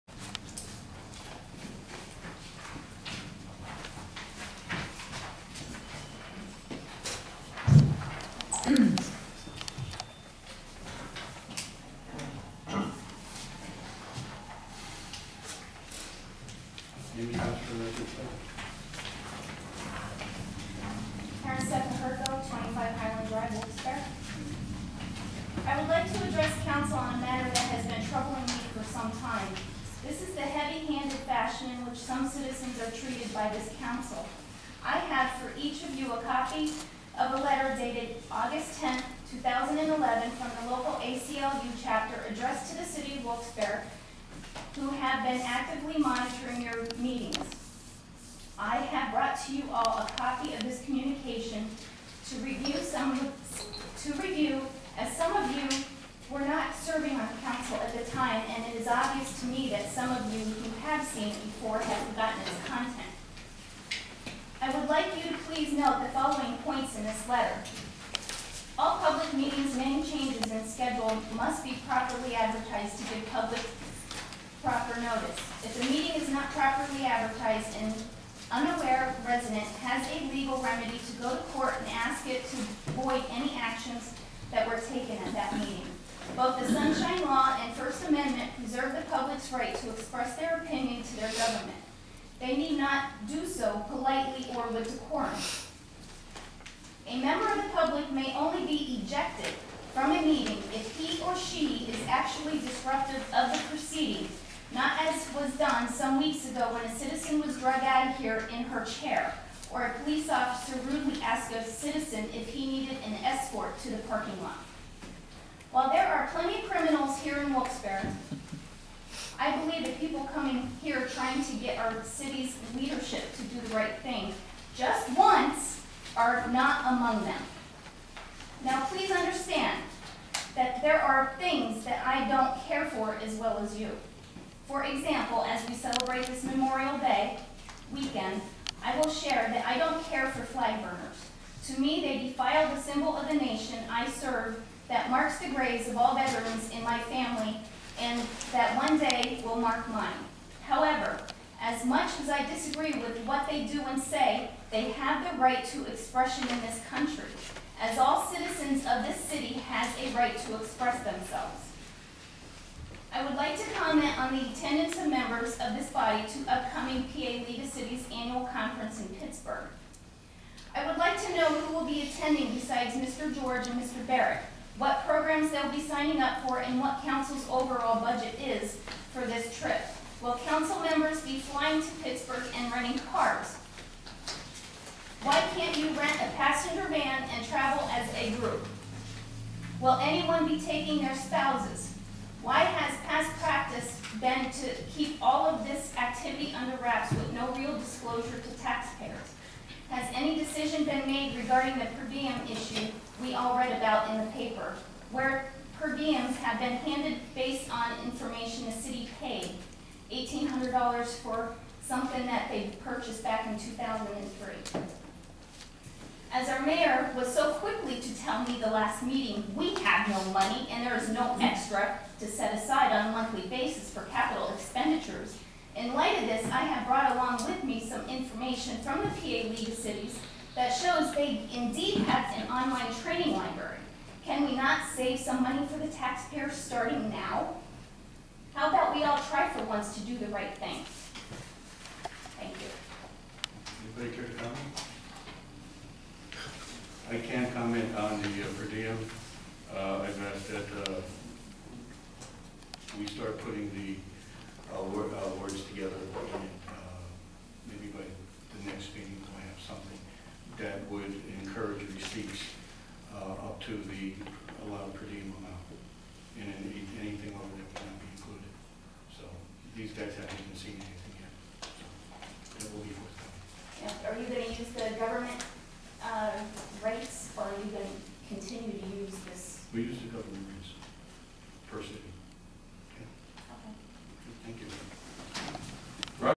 Wilkes-Barre city council meeting 5-24-12 006.MPG
Karen Ceppa Hirko lets the city council know that the ACLU is watching the meetings.